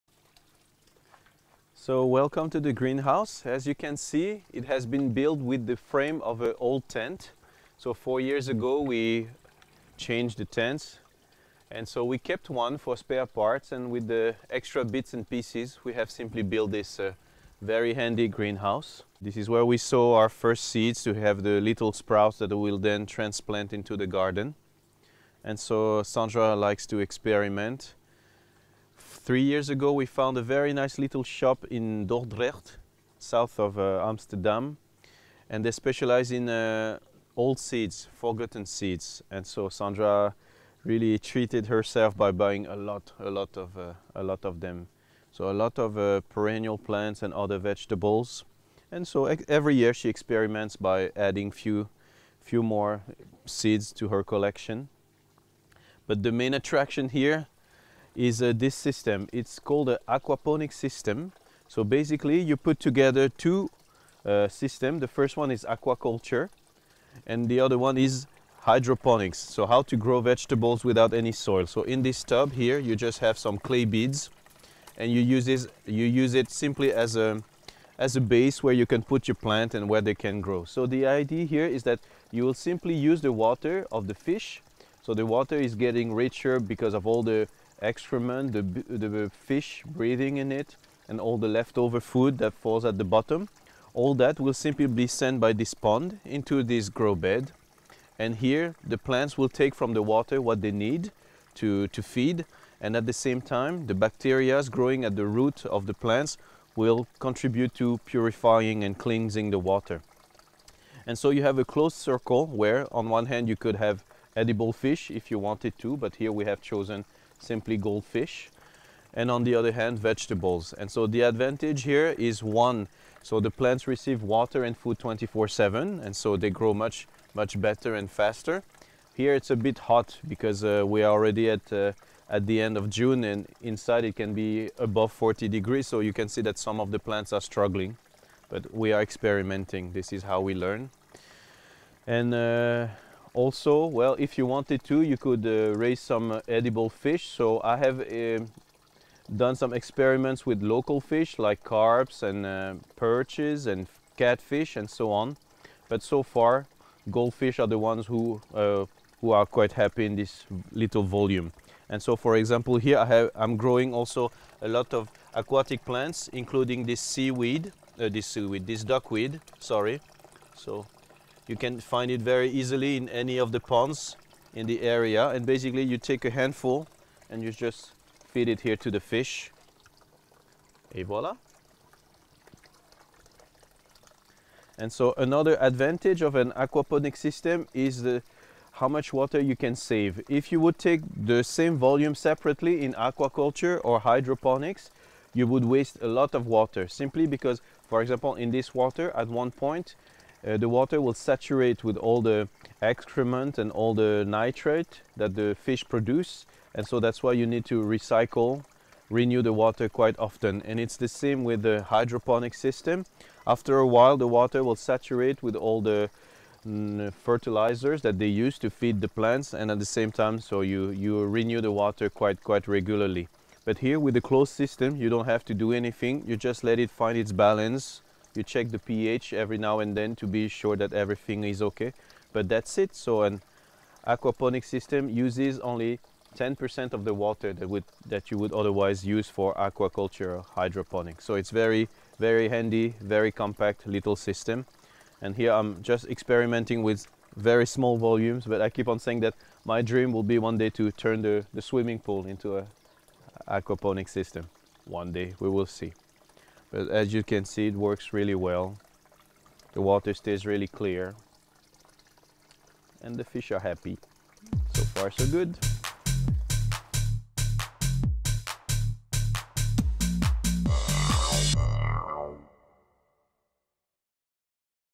Growing Home - Santi show us his aquaponic system
aquaponic system,growing home,sustainable,sustainability